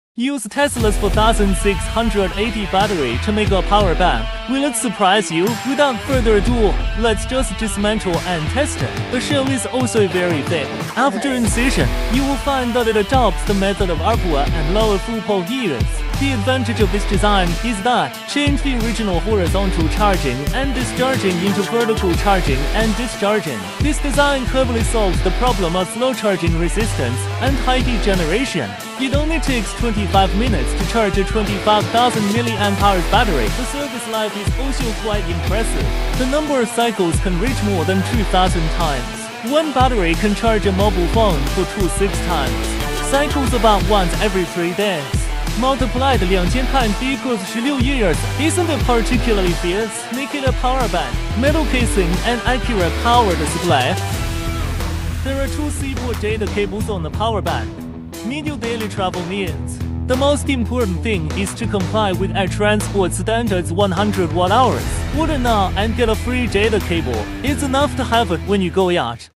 Power bank made of Tesla 4680 batteries
Have you ever seen a power bank made with Tesla 4680 batteries?
This is the finished product! Let’s take a look together.
If you like this creative power bank, you can go to this link to learn more about it.
Here is a link to the same power bank as in the video